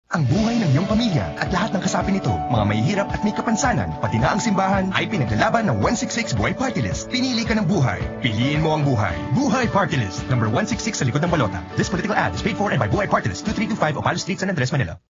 BUHAY Partylist Paid Radio Ad May 2022 15s
This is the Political Radio Ad for BUHAY Partylist Political Party for their race to the Congress this coming May 9, 2022 - as found on number 166 of your ballot (under PARTYLIST / Vote for 1 Section).

DRPS Two 1822: BUHAY Partylist Paid Radio Ad May 2022 15s